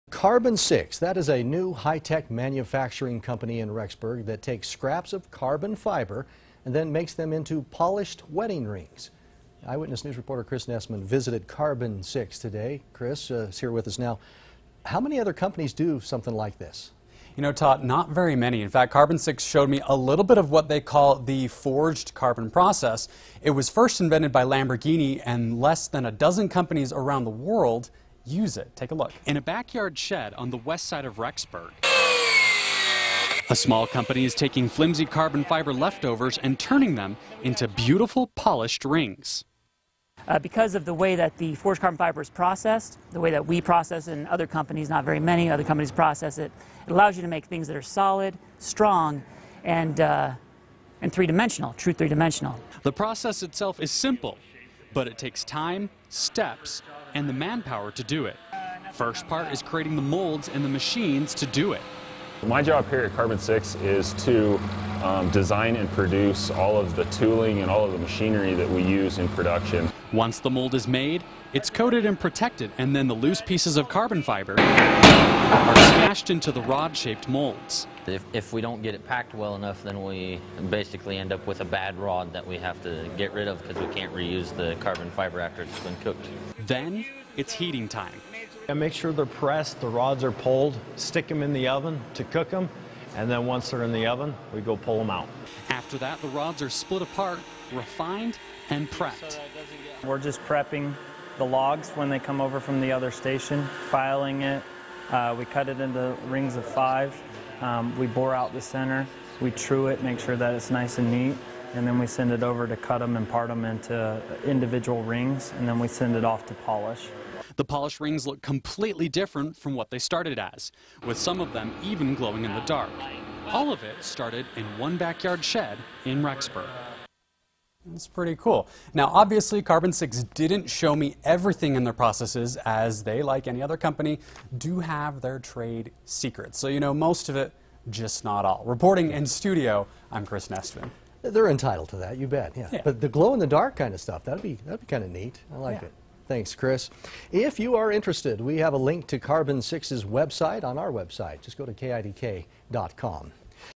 Rexburg business turns carbon fiber scraps into wedding ring
The process was first invented by Lamborghini and less than a dozen companies around the world use it.